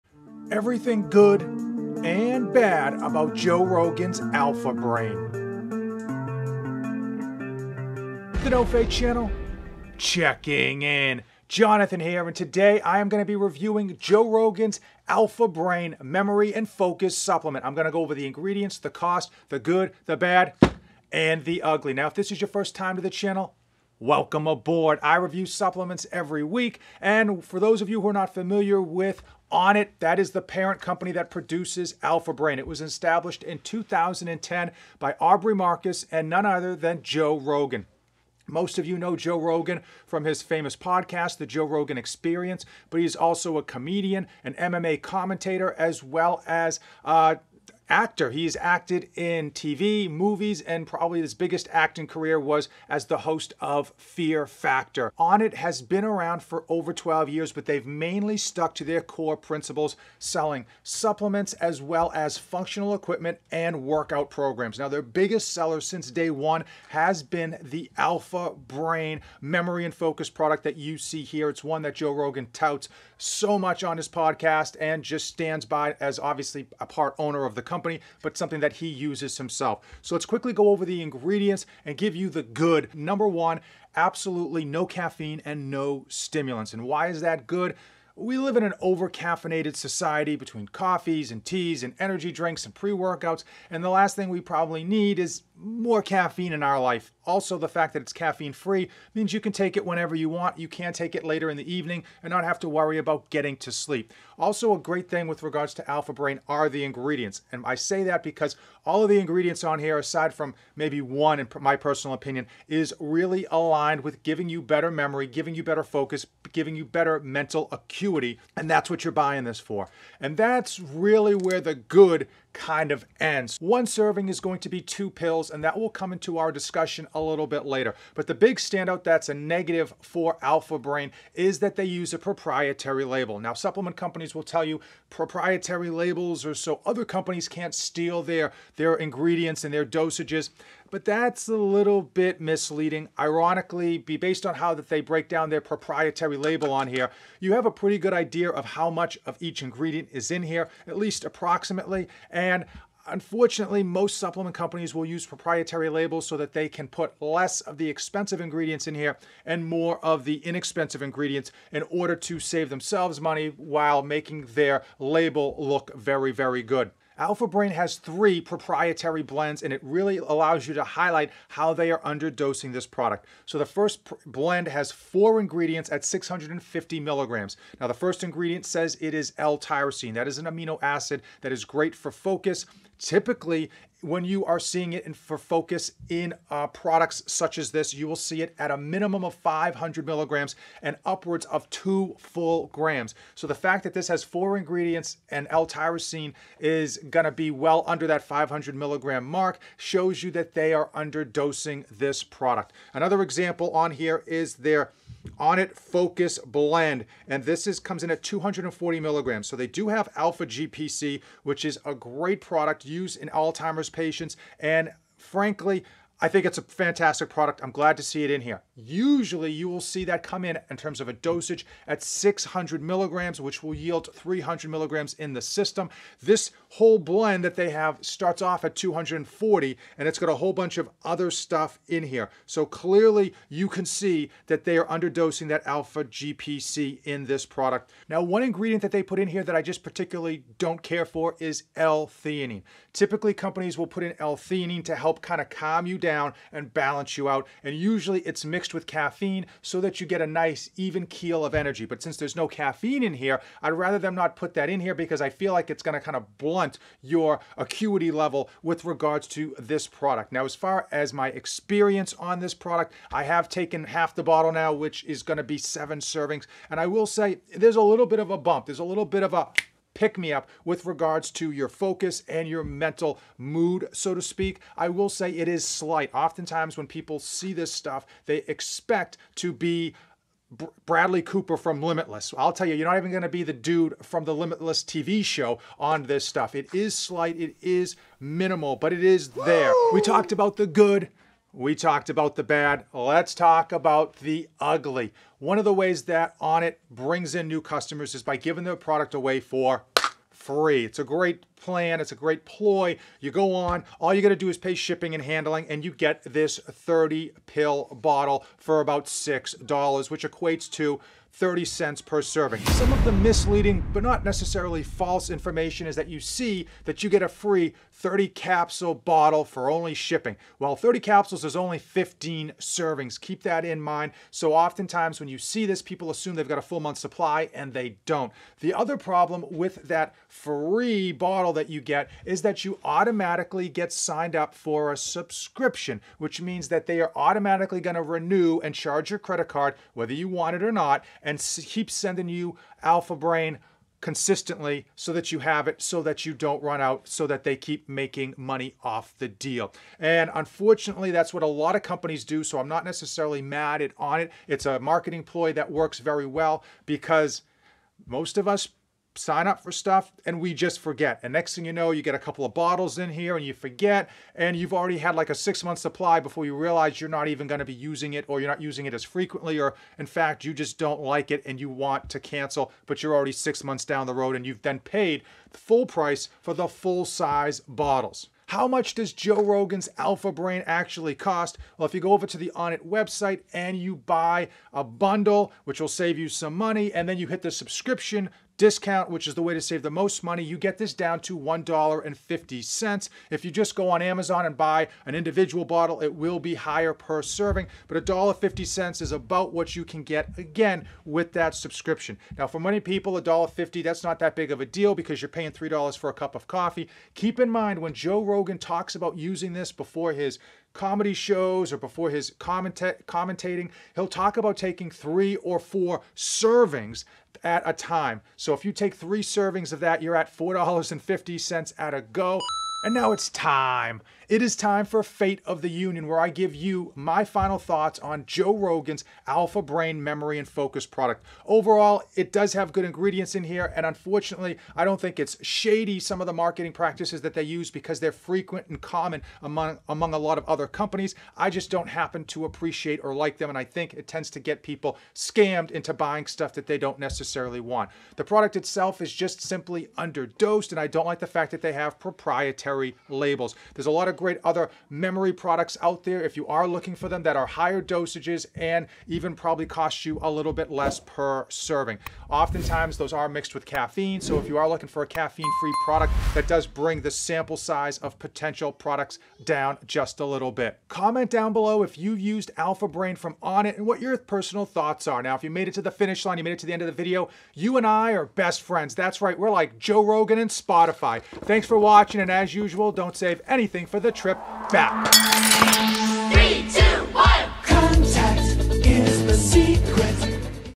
Alpha Brain Review | What Joe Rogan doesn't tell you....
Real truth behind Joe Rogan's Alpha Brain from Onnit...the good the bad and the ugly
Top Nootropic product that's properly dosed
Alpha Brain
Code: NoFate247